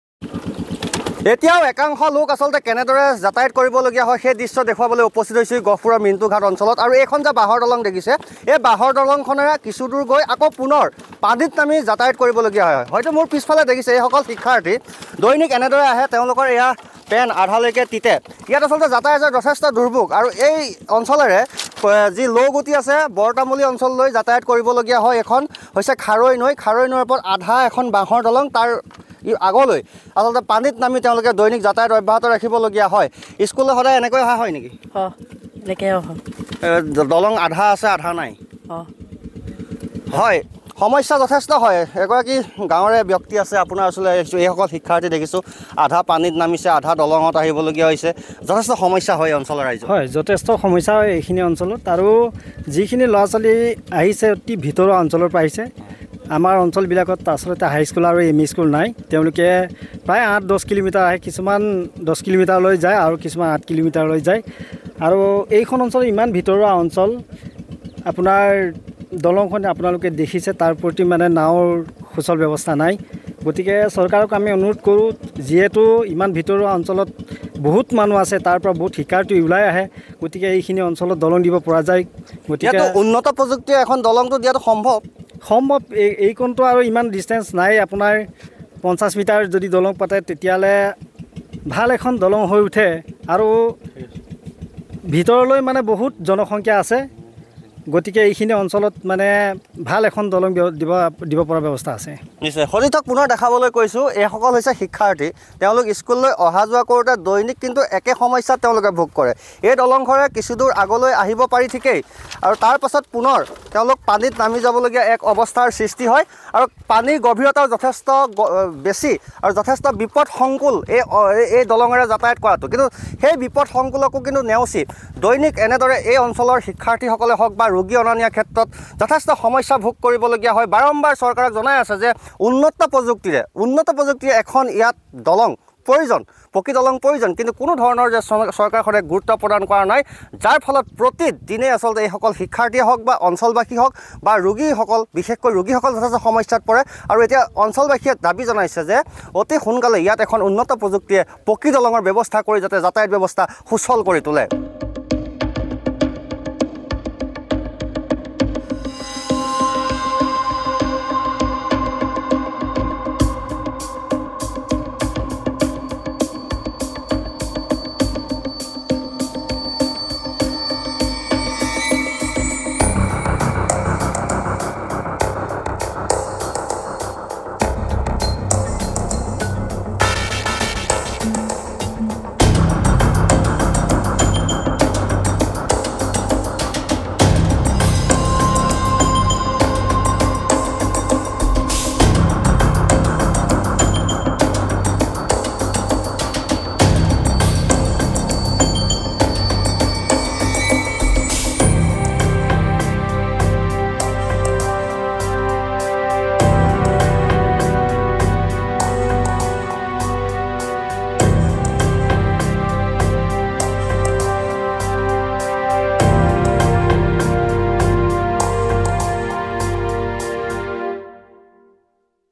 Gohpur Broken Bridge | গহপুৰৰ খাৰৈ নৈৰ ওপৰত অৰ্ধাংশ বাঁহৰ ভগ্ন দলঙেৰে বিপদ সংকুল যাতায়ত | N18V
Gohpur Broken Bridge |  গহপুৰৰ খাৰৈ নৈৰ ওপৰত অৰ্ধাংশ বাঁহৰ ভগ্ন দলং আৰু অৰ্ধাংশ পানীত নামি বিপদ সংকুল যাতায়ত লৌগুটি,বৰতামুলী গাৱঁৰ ৰাইজৰ।দীৰ্ঘ বছৰ ধৰি এনেদৰেই বিপদ সংকুল যাতায়ত প্ৰতিদিনে অব্যাহত ৰাখিছে গাওঁবাসীয়ে।স্কুলীয়া শিক্ষাৰ্থী সকলে আধা দলঙেৰে গৈ আধা পানীত নামি দৈনিক পাৰা পাৰ হ'ব লগীয়া হয়। বছৰ বাগৰি গৈ আছে;তথাপিও উক্ত অঞ্চলত এখন উন্নত প্ৰযুক্তিৰে পকী দলং হৈ নুঠিল। যাৰ ফলত আজিও বহু কষ্ট ভোগ কৰি অঞ্চল বাসীয়ে অব্যাহত   ৰাখিছে দুৰ্বিসহ যাতায়ত।